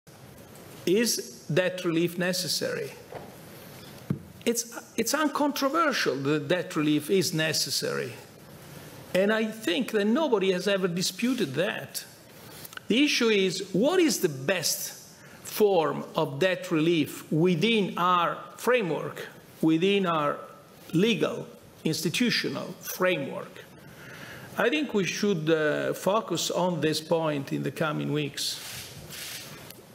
Draghi: “Non c’è dubbio che il debito greco vada alleggerito"
“Non c’è dubbio che il debito greco vada alleggerito”. Ad affermarlo è il governatore della Bce, Mario Draghi, secondo cui “il problema è capire qual è la migliore forma per farlo all’interno del nostro impianto istituzionale”